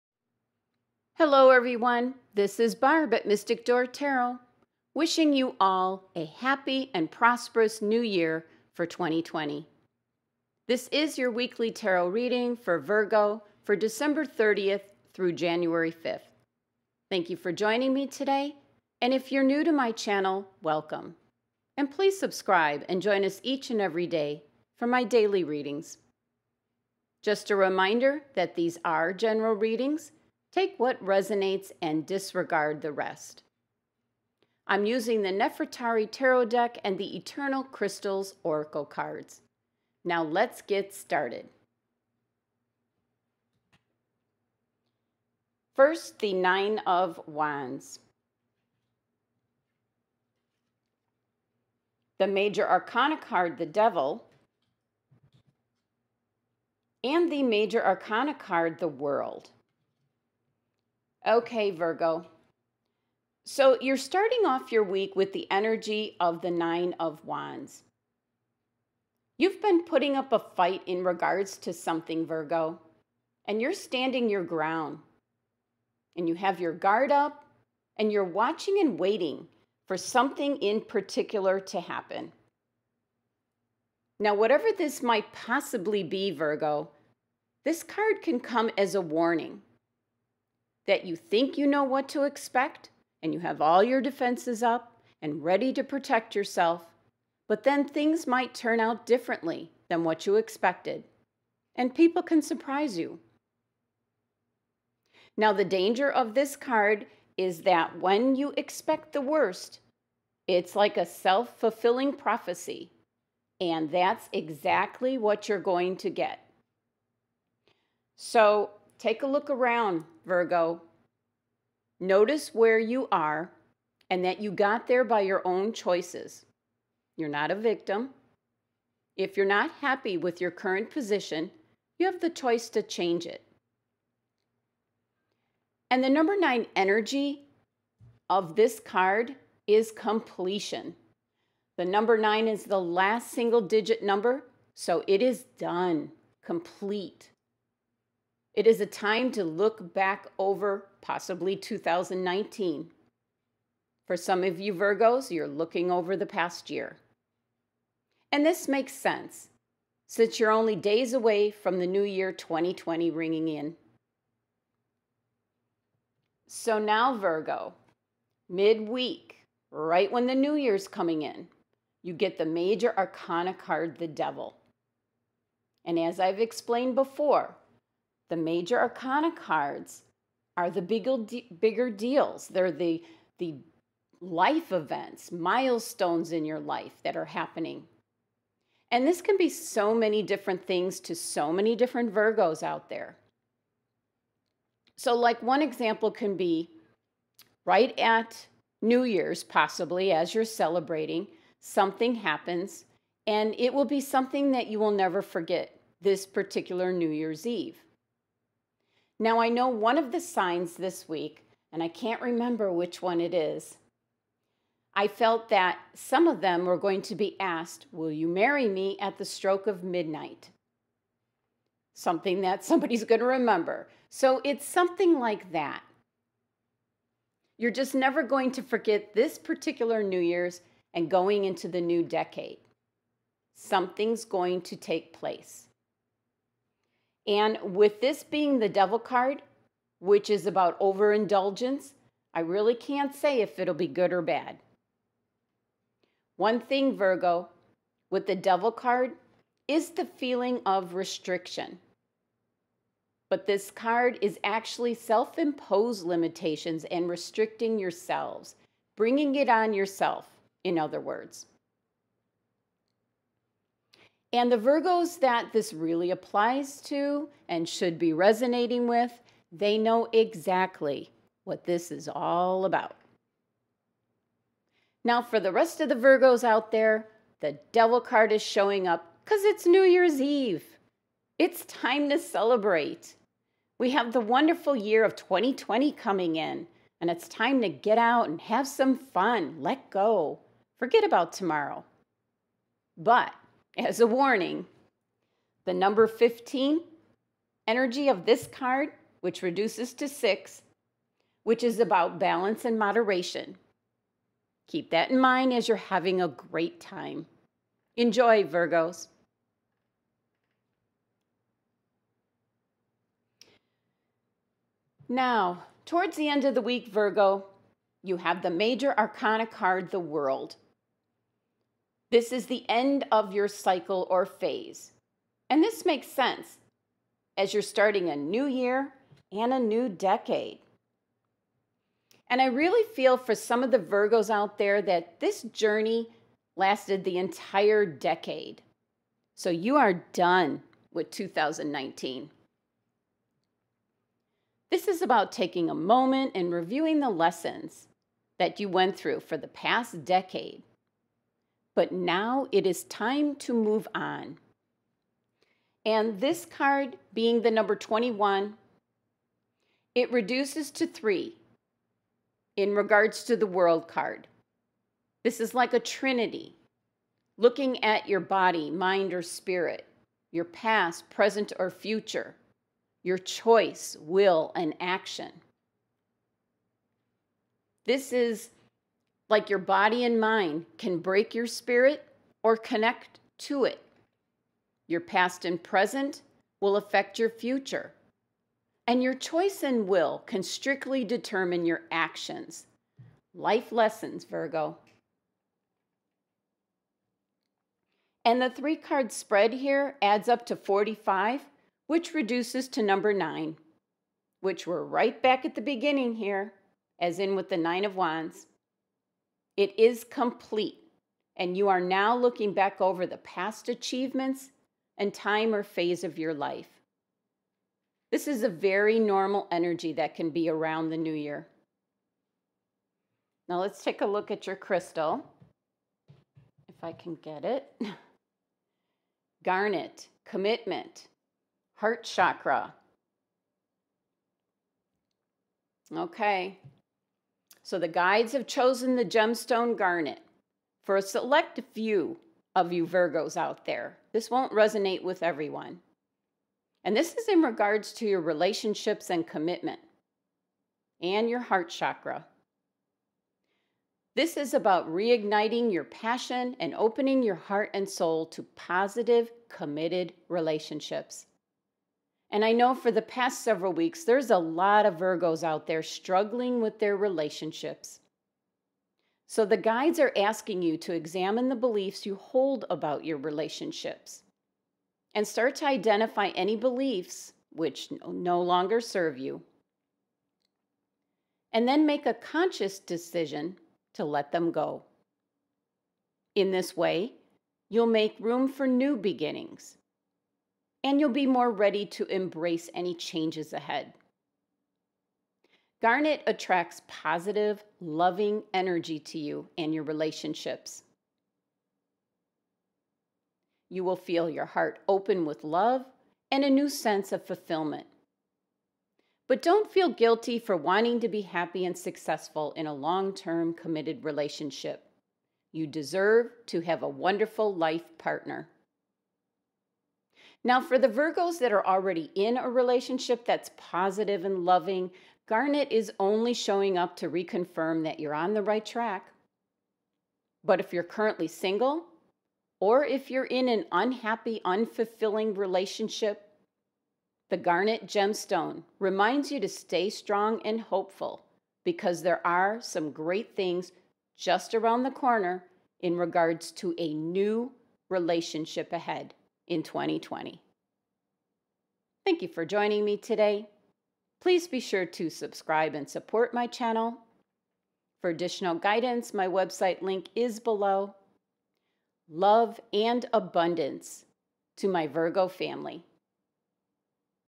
Virgo Weekly Tarot Reading Dec.30-Jan.5 2020 ~ Mystic Door Tarot ~ PARTY LIKE THE DEVIL!
Virgo ♍ ~ Crystal of the Week!  GARNET
This is your weekly tarot reading shedding some light on the week ahead!
Blessings to my Virgo Family! 🙏

**General readings will not resonate with all Virgos.
You would need a reading performed specifically for you Virgo.
Just go to my website for more info.

Nerfertari Tarot Deck - Silvana Alasia
Eternal Crystals Oracle Cards -   Jade-Sky